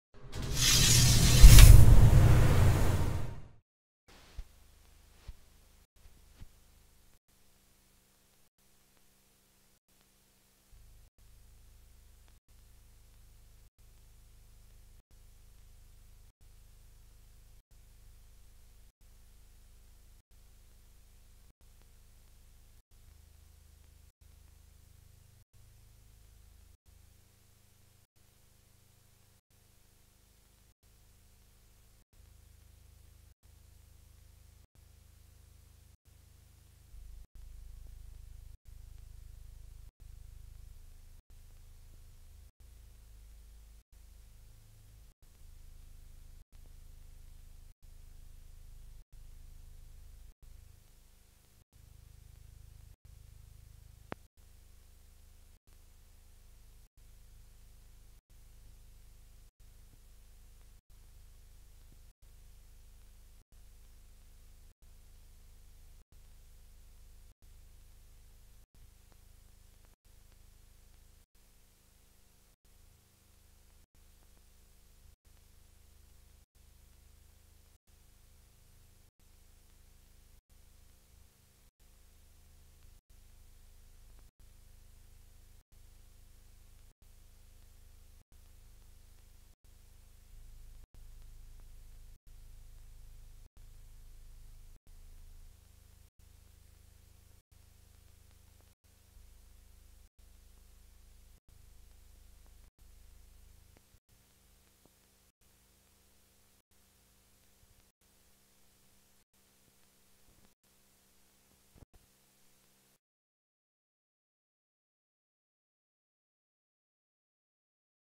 Commuter Airlines event, 1968-05-15
Silent film.; Passengers checking in, boarding and exiting aircraft at Delaware County Airport, Johnson Field followed by an event attended by Muncie and Delaware County government officials. This event appears to be ceremony marking the opening of Commuter Airlines service to Delaware County, Indiana.